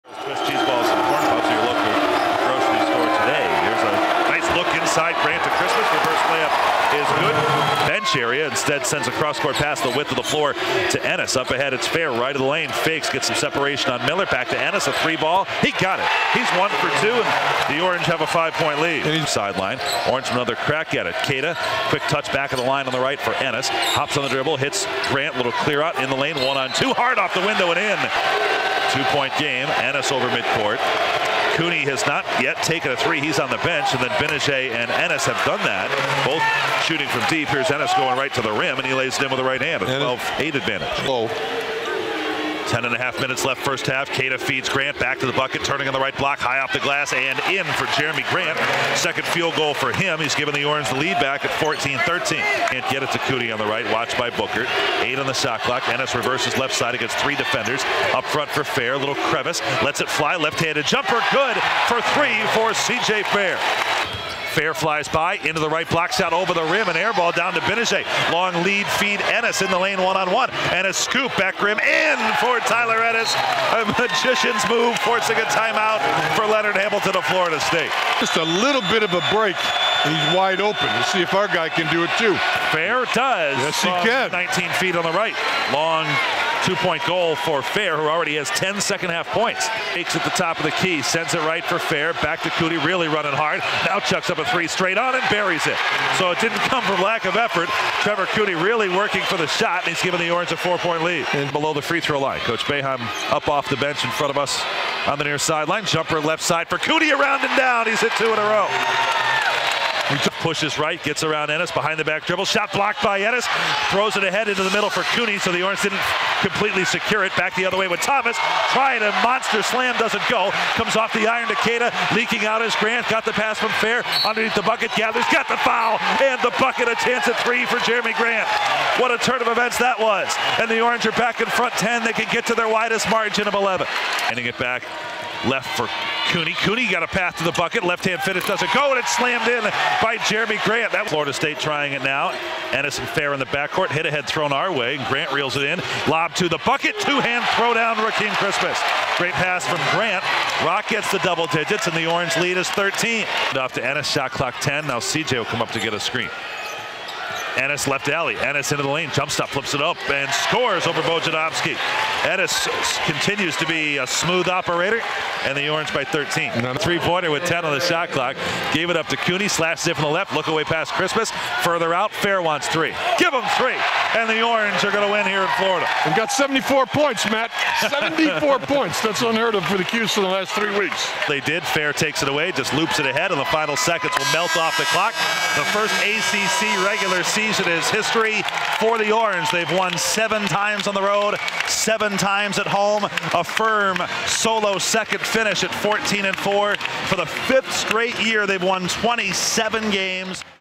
Syracuse vs. Florida State Highlights - Syracuse Men's Basketball
Highlights from Syracuse's 74-58 win at Florida State.